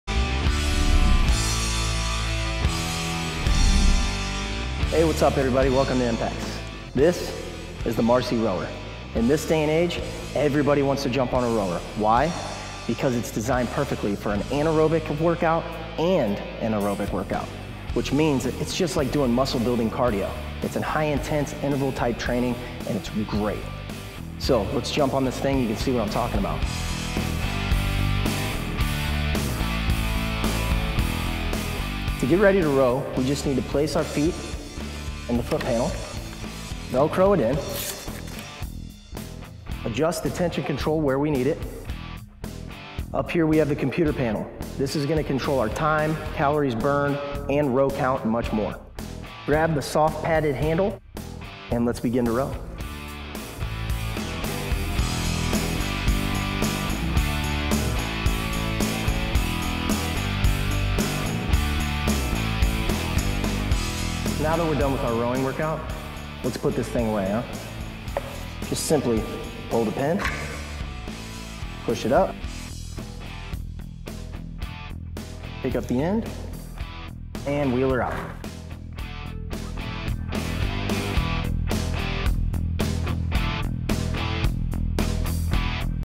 Marcy NS-40503RW - Best Magnetic Resistance Rowing Machine Under $300
► Marcy NS-40503RW Foldable 8-Level Magnetic Resistance Rowing Machine with Transport Wheels
★ US Prices
 This compact rower features 8 levels of resistance to challenge users of all levels of fitness. The ergonomically designed contoured foam seat pad ensures you stay comfortable throughout your entire workout and the foam-covered handles are anti-slip so you can focus on your routine.

Made to accommodate a wide range of users, the multi-angle monitor and adjustable foot pedal straps provide a customizable fit. The quick-release pin and foldable monitor make it easy to fold up the rower and lock it into position for storing in a more confined position; talk about a space saving-design! The built-in transport wheels make it easy to move the rower from room-to-room or to your closet for storage when not in use.

Features
- EIGHT PRESET LEVELS: The Marcy Rowing Machine has a resistance knob that features eight preset magnetic levels that can be customized to match your target workout zone. If you’re casually working out, turn the easy-to-use knob to the lowest level for high-speed rowing

- GREAT CARDIO WORKOUT: Burn fat and tone your muscles every time you go for a spin on this exceptional training equipment. It will help you increase your cardiovascular endurance and develop a well-defined upper and lower body.

- ERGONOMIC DESIGN: This rowing machine has user-friendly parts that offer accessibility, functionality, and efficiency. The two conveniently placed pedals have a slip-resistant design for safety and comfort, plus its customizable foot straps accommodate people of all sizes. This rowing utility’s row handles are foam-covered to prevent slipping, and its foam seat pad is contoured to keep you comfortable throughout your routine.

- MULTI-ANGLE MONITOR: Thismachine comes equipped with a multi-angle monitor that folds both forwards and backwards. It’s an easy-to-read LCD panel that provides useful information regarding workouts.

- COMPACT AND FOLDABLE: This workout device features an innovative foldable design that allows you to store it conveniently, including a quick-release pin and a foldable monitor that locks into position. It also has two built-in transport wheels that allow easy moving and storage when you’re not using the machine.